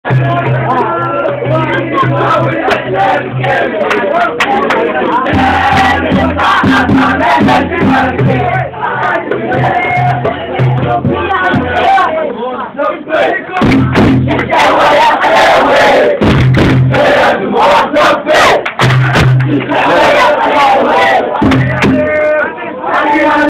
bouznika blue killers
déplacement du peule wifa9 vers la ville de ben slimane drari ZWinen dyal bzk